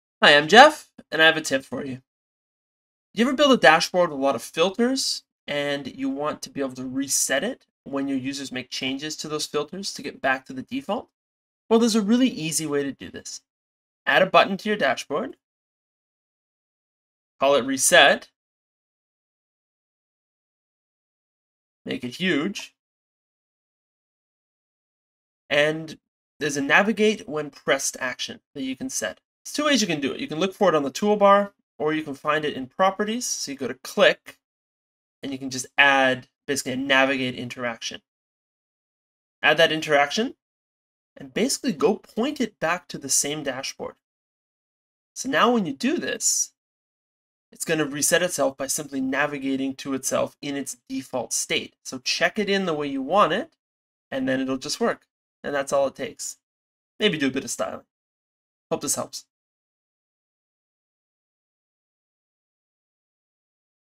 Dundas BI Tip 17 : How-to Add Button to Reset Filters to Default
Sometimes you will have many filters on a dashboard, and you might want to give your users a way to reset them.  Resetting a dashboard will allow your users to revert it back to its initial view state.  This video will show you an easy way to add a reset button to your content without writing code in Dundas BI.

If you'd like to learn more, I'd recommend you go to my learning channel - Off the Charts (with Jeff).